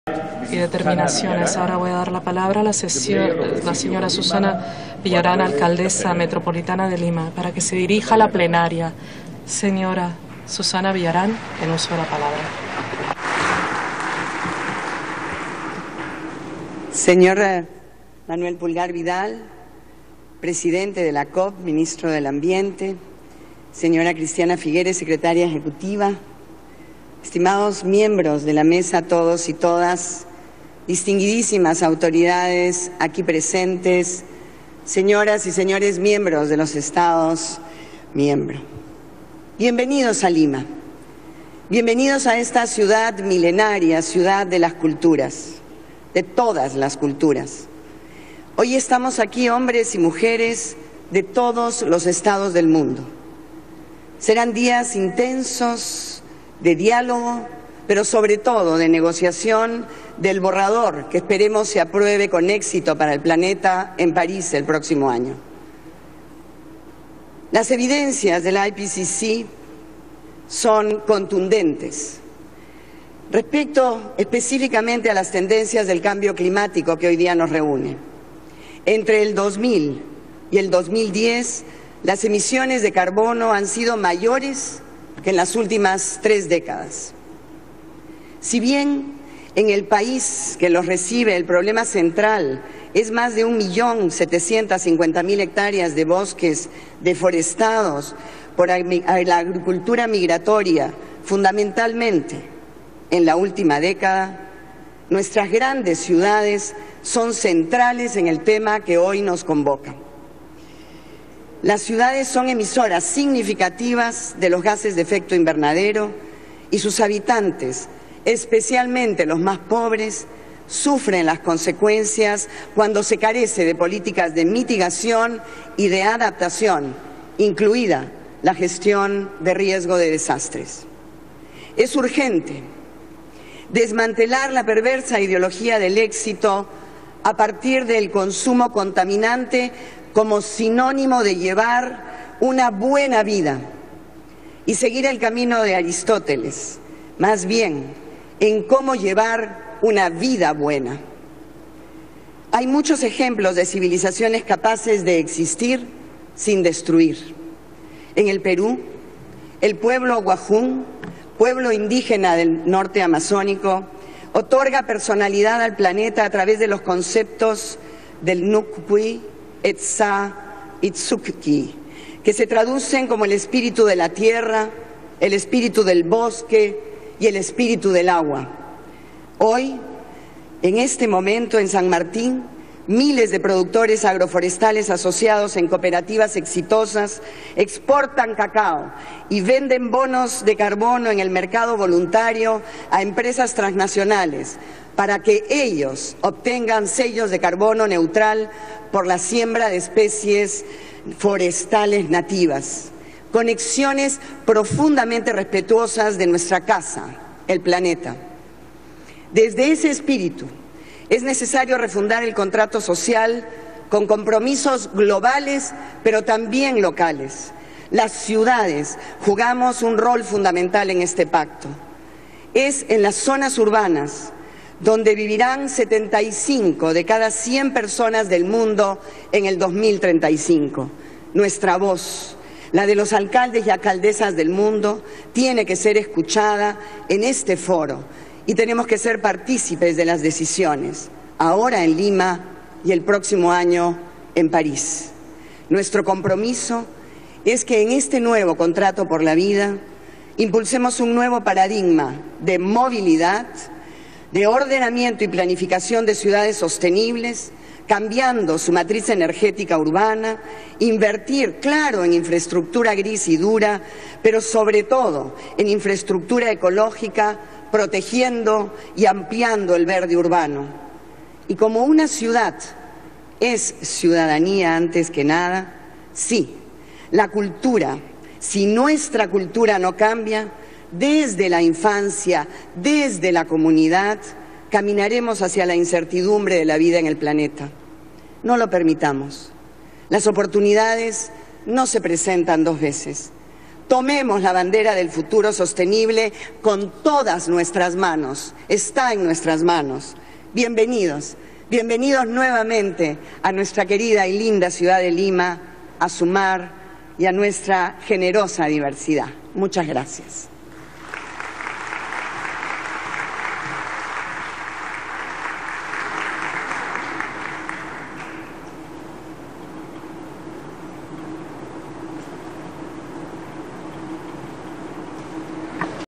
Discurso de la alcaldesa de Lima Susana Villarán en la inauguración de la COP 20
Durante su discurso, señaló que hay muchos ejemplos de civilizaciones capaces de existir sin destruir, nombró como una de ellas al pueblo Awajún y a los productores de cacao de la región San Martín. Dijo también que si la educación en materia del medio ambiente no cambia se caminará rumbo a la incertidumbre.